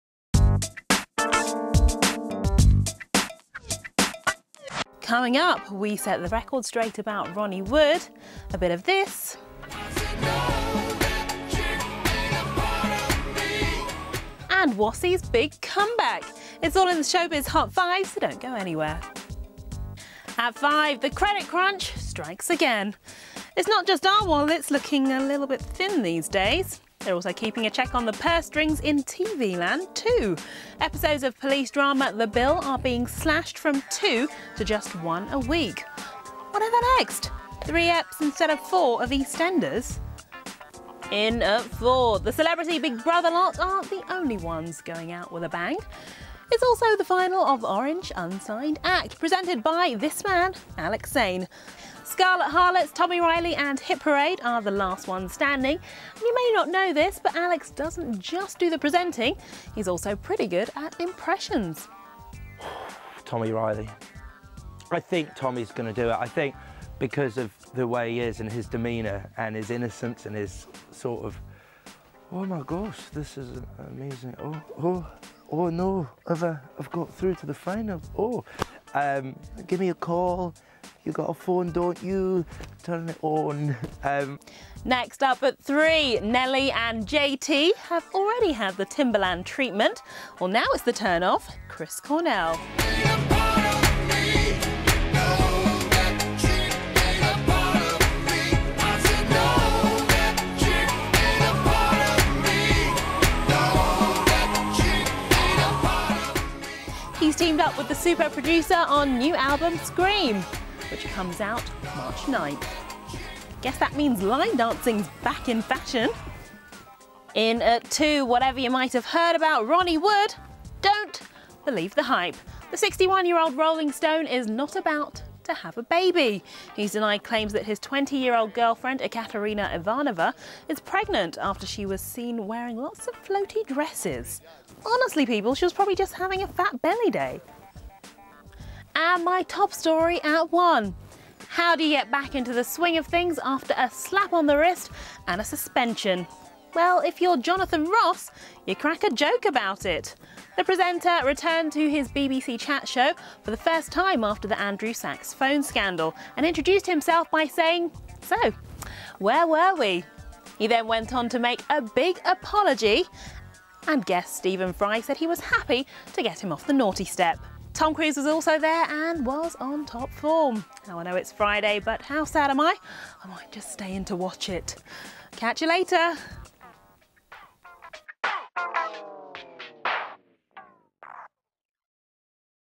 Jonathan Ross's comeback, Ronnie wood denies rumours
BBC chat show host bounces back - Rolling Stone slams baby claims - Timbaland's new track with Chris Cornell Test